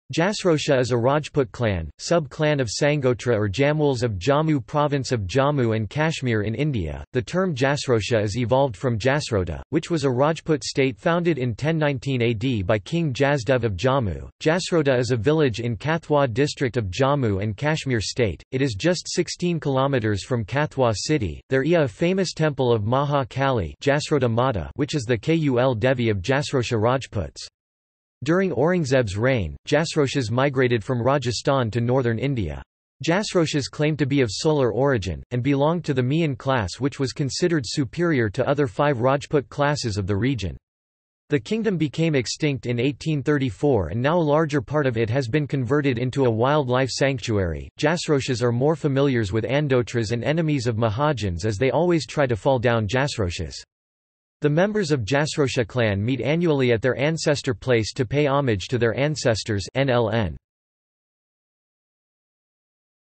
Jasrotia | Wikipedia audio article
This is an audio version of the Wikipedia Article:
Jasrotia

- Socrates

SUMMARY
Jasrotia is a rajput clan, sub-clan of Sangotre or Jamwals of Jammu province of Jammu & Kashmir in India.The term Jasrotia is evolved from Jasrota, which was a rajput state founded in 1019 A.D. by King Jasdev of Jammu.Jasrota is a village in Kathua District of Jammu And Kashmir state.It is just 16km from Kathua city.There ia a famous temple of Maha Kali(Jasrota Mata) which is the Kul Devi of Jasrotia Rajputs. During Aurangzeb's reign, Jasrotias migrated from Rajasthan to northern India. Jasrotias claimed to be of solar origin, and belonged to the Mian class which was considered superior to other five rajput classes of the region. The kingdom became extinct in 1834 and now a larger part of it has been converted into a wild-life sanctuary.Jasrotias are more familiars with Andotras and enemies of Mahajans as they always try to fall down jasrotias. The members of  Jasrotia clan meet annually at their ancestor place to pay homage to their ancestors {NLN}.